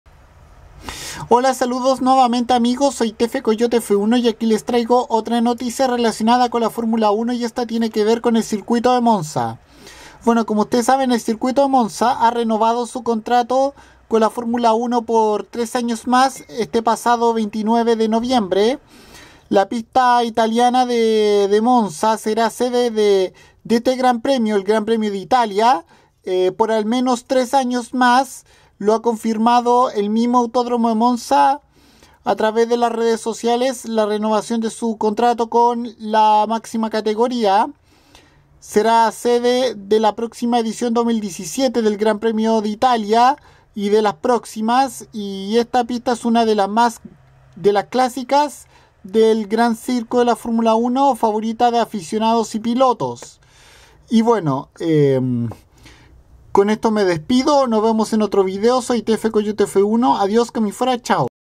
Monza renueva su contrato con la F1 por 3 años mas
La pista italiana de Monza seguirá siendo sede del GP de Italia por 3 años mas sobretodo de 2017 en el calendario de la F1